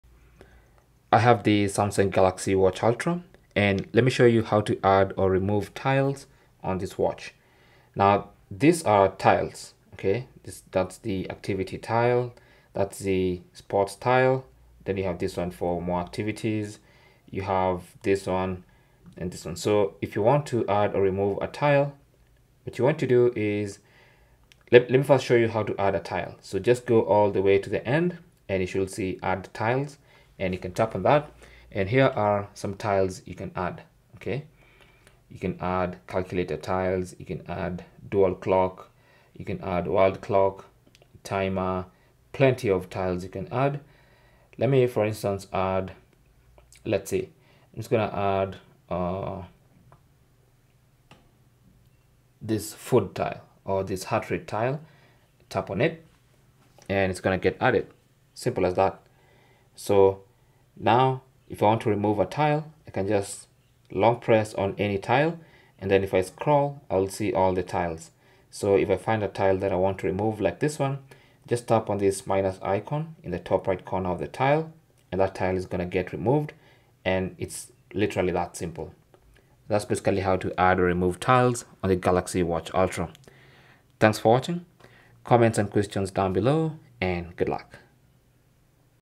How To Add / Remove Tiles On Samsung Galaxy Watch Ultra
Our brief guide to show you how to add / remove tiles on your Samsung Galaxy Watch Ultra. Customize your Galaxy Watch Ultra tiles.

If you're tired of having too many tile pages on your watch, you can remove tiles that you no longer want on there. However, you can always add as many tiles as you'd like, or re-add ones that you had removed before.

Add or Remove Tiles On Samsung Galaxy Watch Ultra:

How To Add Tiles On Samsung Galaxy Watch Ultra:

Step 1: To add tiles on your Galaxy Watch Ultra, swipe right or left from your watch face until you get to the "+ Add Tiles" option. Tap on it. You will be presented with a couple of tile options you can add.

Step 2: Scroll through and select the tile you want to add and tap on it. The tile will be added to the tile order on your Galaxy Watch Ultra.

How To Remove Tiles On Samsung Galaxy Watch Ultra:
How To Delete Tiles On Samsung Galaxy Watch Ultra:

Step 1: To remove a tile, swipe right or left and find the tile you want to remove.

Step 2: Tap and hold the tile to enter edit mode. A "-" icon should appear on the tile. Tap on the "-" icon and the tile will be removed from the order of tiles.

SAMSUNG Galaxy Watch Ultra 47mm LTE AI Smartwatch

Cell Phone Tripod Adapter